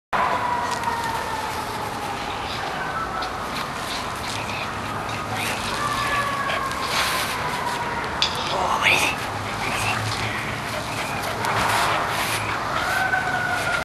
Fabiola is fabulous!
Fabiola is a fun and loving pit bull mix available at San Francisco Animal Care & Control